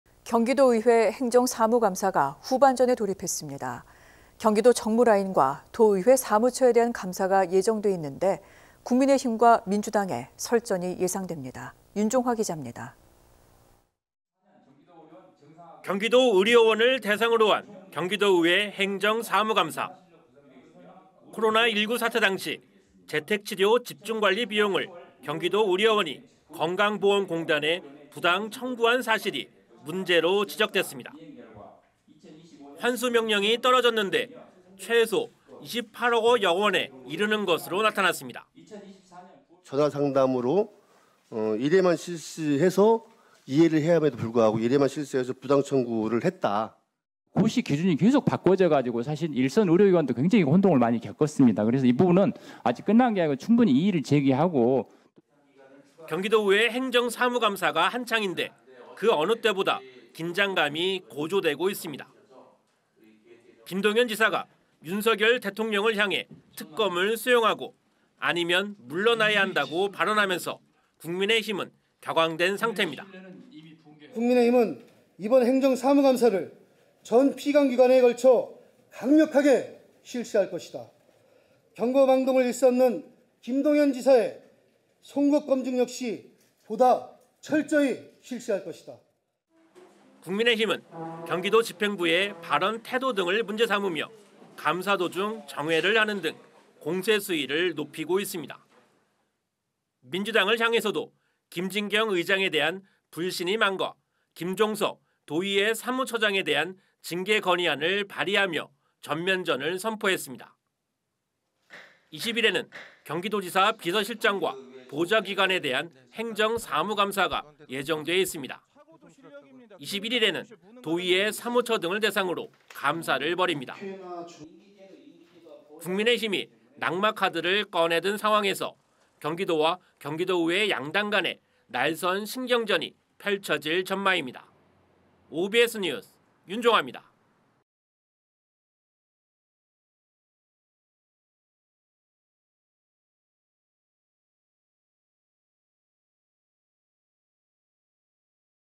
경기도의회 감사 후반전..도 정무라인 두고 설전 예고
【앵커】
경기도의회 행정사무감사가
후반전에 돌입했습니다.
경기도 정무라인과
도의회 사무처에 대한 감사가
예정돼 있는데,
국민의힘과 민주당의
설전이 예상됩니다.
윤종화 기자입니다.

【기자】

경기도의료원을 대상으로 한 경기도의회 행정사무감사.

코로나19 사태 당시 재택치료 집중관리 비용을 경기도의료원이 건강보험공단에 부당 청구한 사실이 문제로 지적됐습니다.

환수 명령이 떨어졌는데, 최소 28억여 원에 이르는 것으로 나타났습니다.

[최만식 / 경기도의회 더불어민주당 의원(지난 13일): 전화상담으로 1회만 실시해서 2회를 해야함에도 불구하고 1회만 실시해서 부당청구를 했다….]

[이필수 / 경기도의료원장(지난 13일): 고시 기준이 계속 바뀌어서 일선 의료기관이 굉장히 혼동을 겪었습니다. 이 부분은 아직 끝난게 아니고 충분히 이의제기를 하고….]

경기도의회 행정사무감사가 한창인데 그 어느 때보다 긴장감이 고조되고 있습니다.

김동연 지사가 윤석열 대통령을 향해 특검을 수용하고, 아니면 물러나야 한다고 발언하면서 국민의힘은 격앙된 상태입니다.

[김정호 / 경기도의회 국민의힘 대표의원(지난 13일): 국민의힘은 이번 행정사무감사를 전 피감기관에 걸쳐 강력하게 실시할 것이다. 경거망동을 일삼는 김동연 지사의 송곳검증 보다 철저히 실시할 것이다.]

국민의힘은 경기도 집행부의 발언 태도 등을 문제 삼으며 감사 도중 정회를 하는 등 공세 수위를 높이고 있습니다.

민주당을 향해서도 김진경 의장에 대한 불신임안과 김종석 도의회 사무처장에 대한 징계건의안을 발의하며 전면전을 선포했습니다.

20일에는 경기도지사 비서실장과 보좌기관에 대한 행정사무감사가 예정돼 있습니다.

21일에는 도의회 사무처 등을 대상으로 감사를 벌입니다.

국민의힘이 낙마 카드를 꺼내든 상황에서 경기도와 경기도의회 양당간에 날선 신경전이 펼쳐질 전망입니다.

OBS뉴스 윤종화입니다.